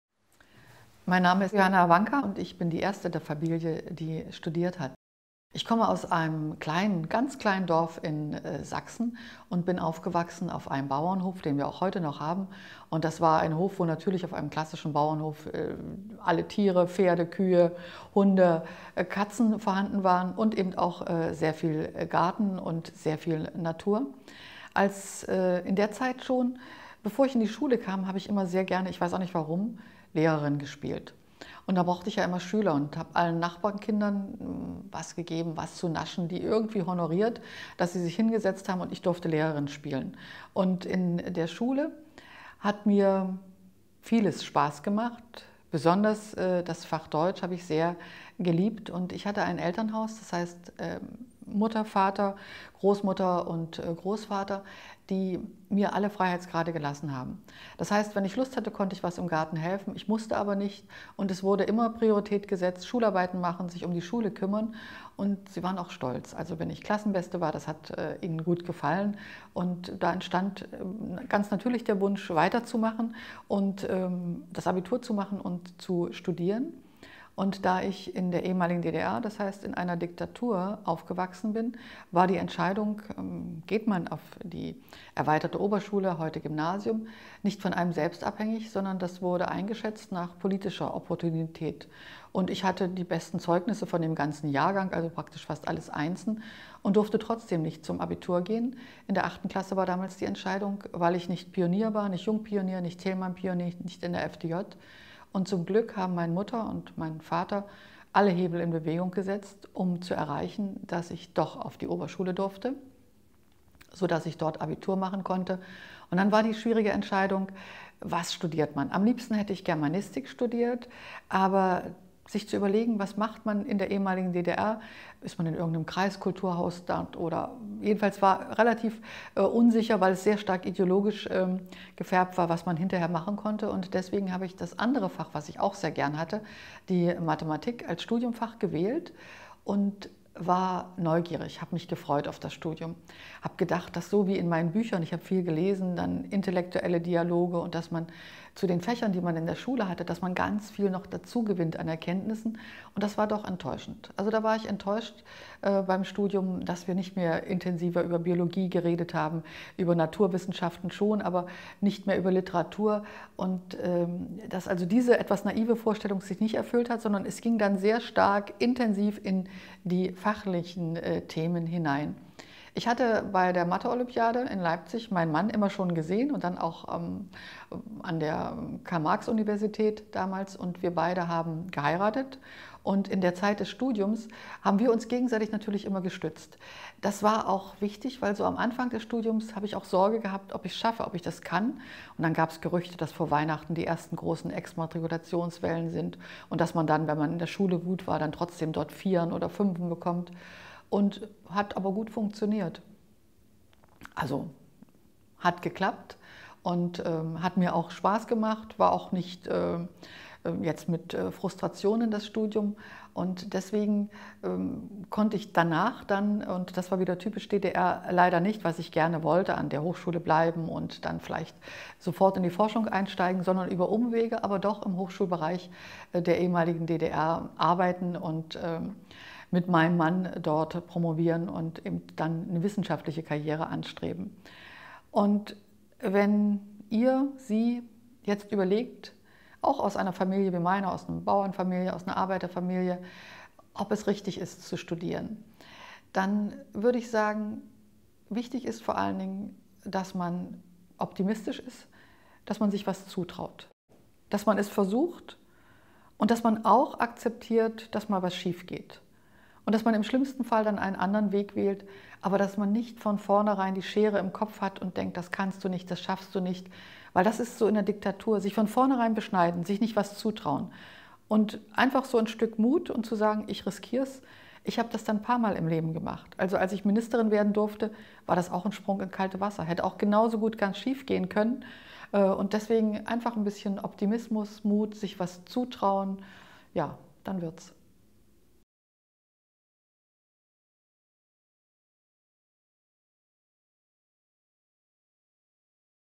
Prof. Dr. Johanna Wanka, Deutsche Politikerin (CDU), für
Die ehemalige Bundesbildungsministerin Johanna Wanka erzählt für die Videokampagne "Erste an der Uni", dass sie als erste in ihrer Familie studiert hat und macht Mut zum Studium.